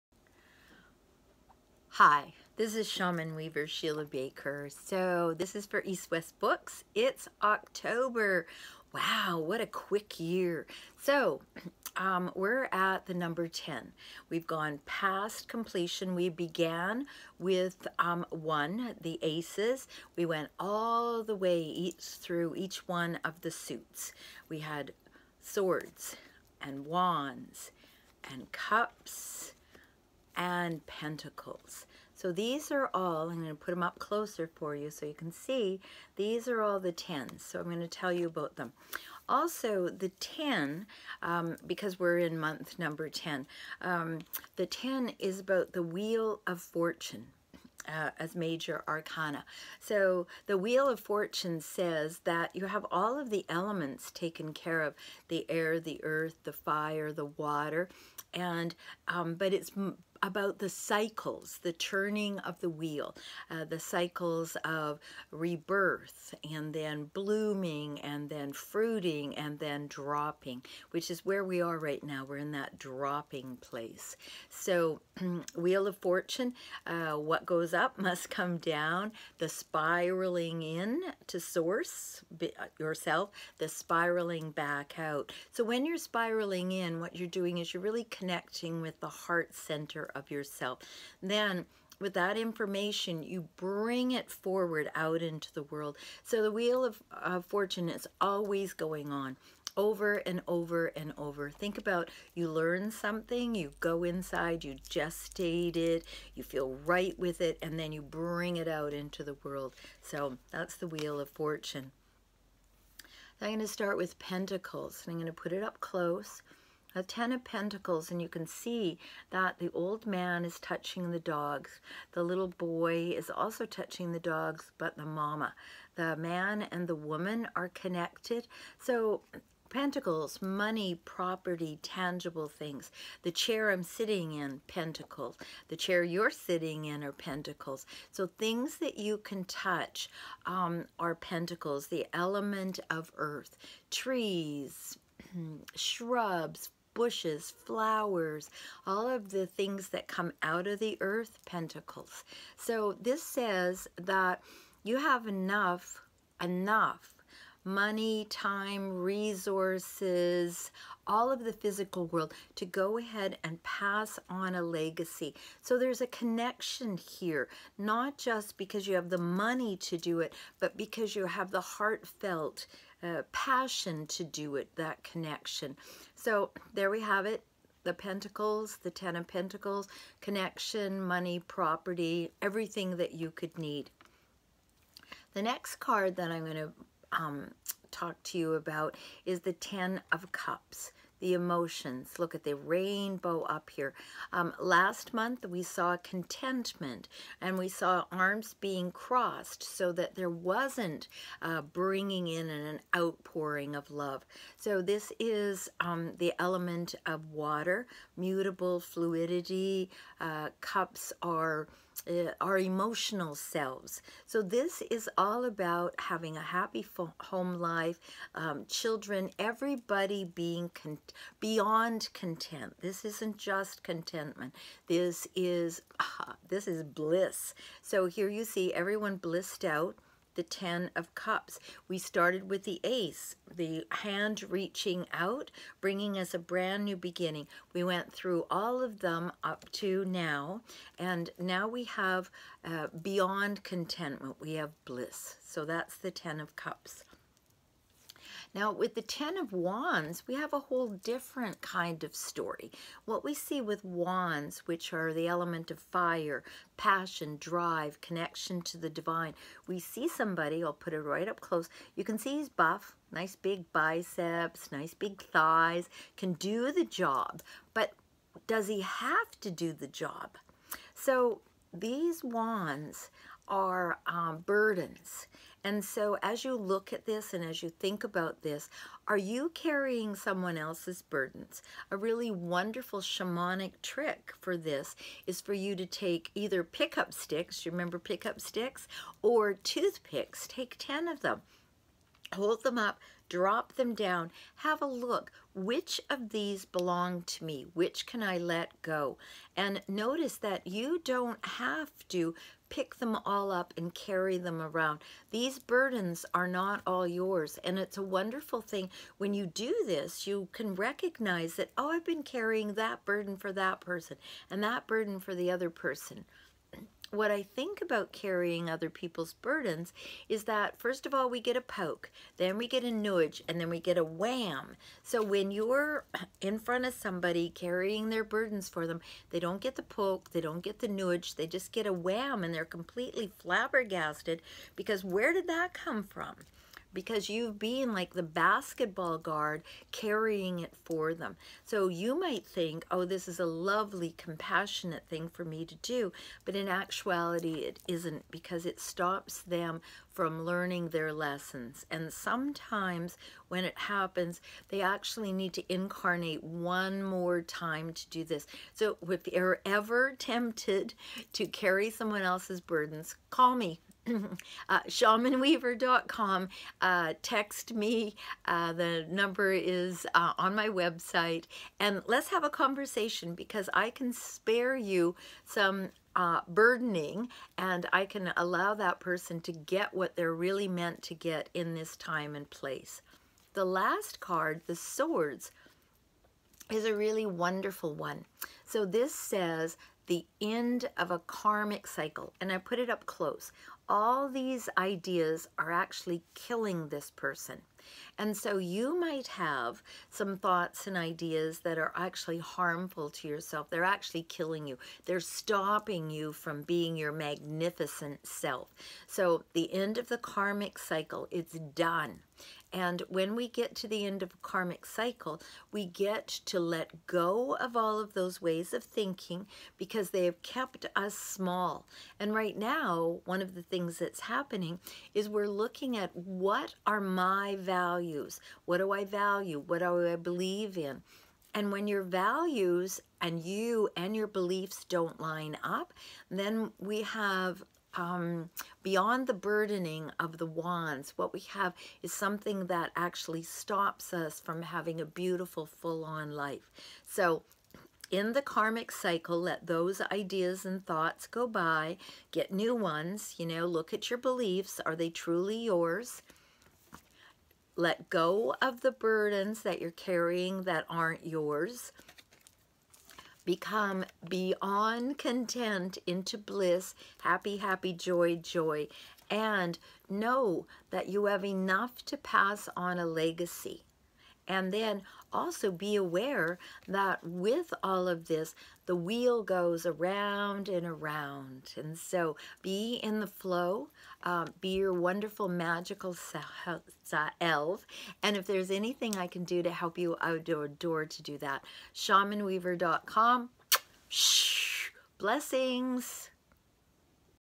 EWB Tarot Card Blog & Reading for October 2017
"As the days get shorter and the nights grow longer we turn inward to comfort- comfortable emotions as in the Cups; physical world reality as in the Pentacles; thoughts as in the Swords; and spiritual connections as in the Wands..
The TEN of each of the Tarot suits, Cups , Pentacles, Swords and Wands bring closure. With the 10 of Cups comes emotional fulfillment - no . longer striving to connect; 10 of Pentacles being able to pass along a legacy; 10 of Swords - the end of a Karmic cycle; 10 of Wands dropping others burdens!
What a great month! As you watch the leaves fall let ALL that no longer serves YOU drop away."
--Shamanweaver Shiela Baker

Shiela does private consultations at East West Bookshop every Thursday!